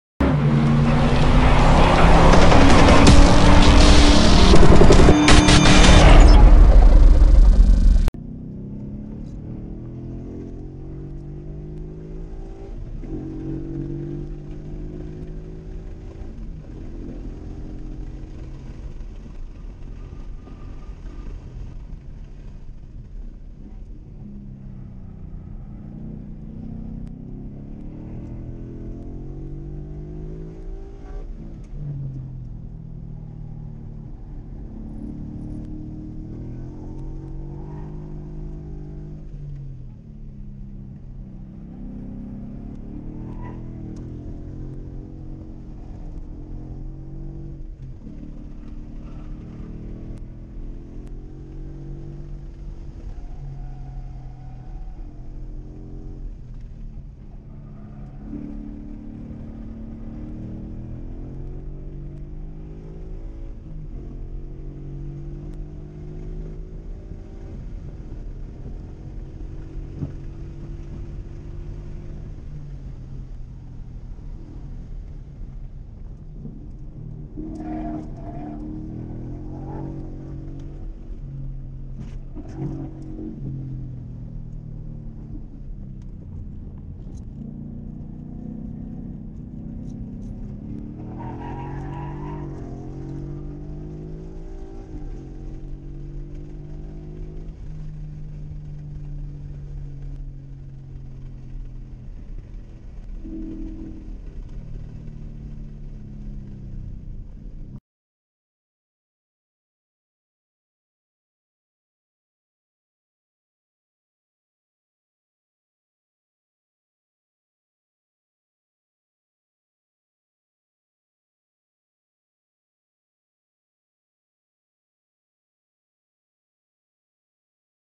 2015 Ford Mustang GT @ Streets of Willow 1:21.551
As I continue to modify and change the setup on my 2015 Ford Mustang GT I continue to gather data on the car and continue to lower lap times. This time with a new Personal Best at Streets of Willow CW. My previous best was a 1:22.850. This was a substantial time drop and I think with some adjustments to the camber on the rear of the car it could get even better.